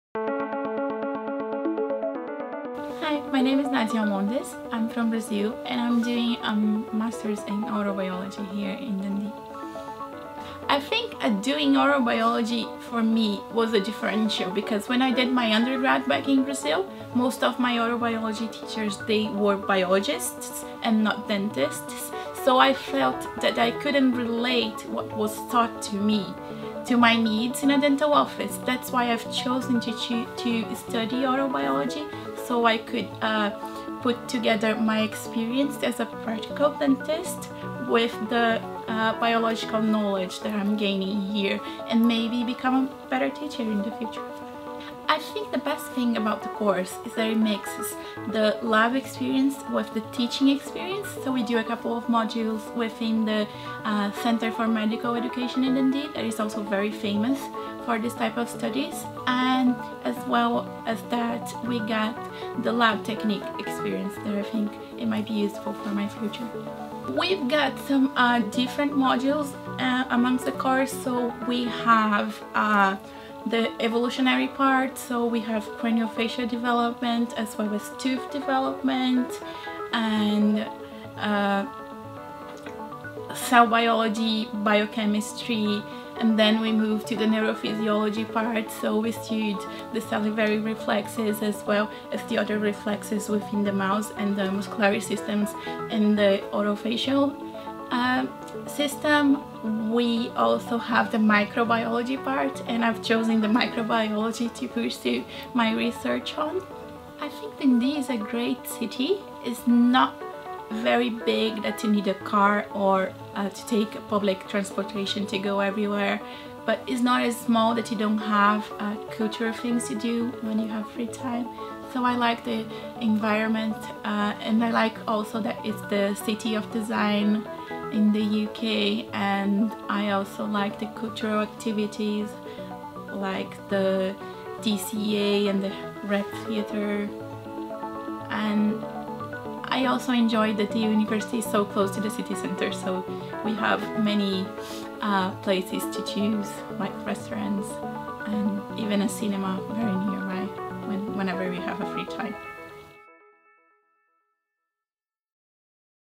University of Dundee | Dentistry | MSc Oral Biology
Nadja Almondes speaks about her experience in Oral Biology at the University of Dundee.

Scottish University of the Year 2016, Times Good University Guide 2016